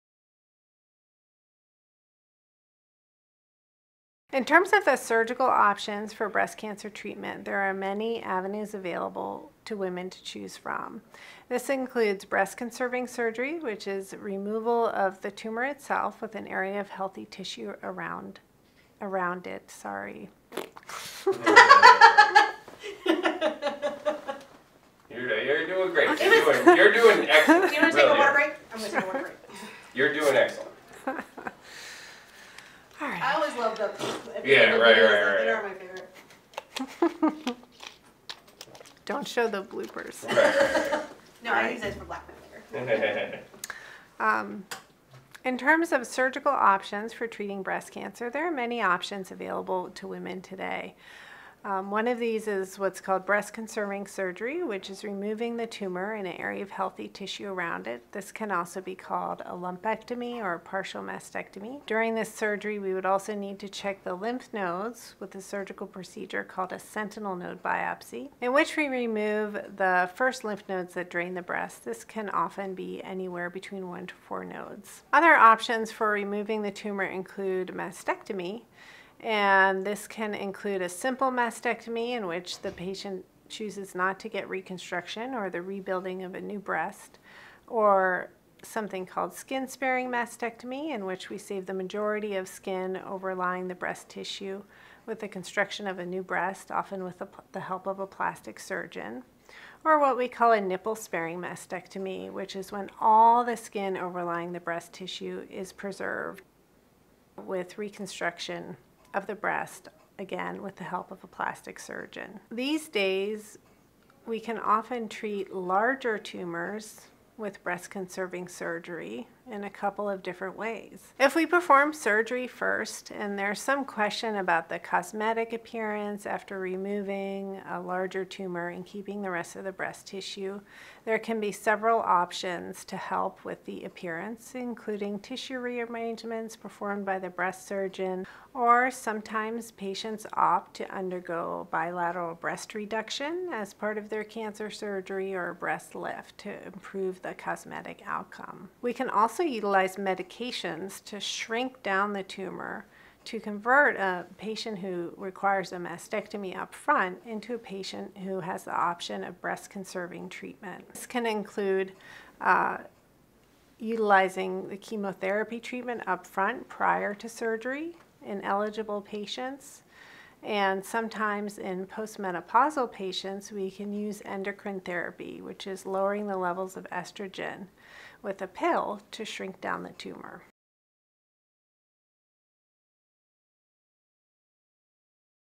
Breast Cancer - Surgical Options
When there is a diagnosis of breast cancer, surgical options may be available. Johnston-Willis Hospital’s surgical oncologist, Dr. Emily Bellavance, outlines what those options may be and how to proceed with treatment.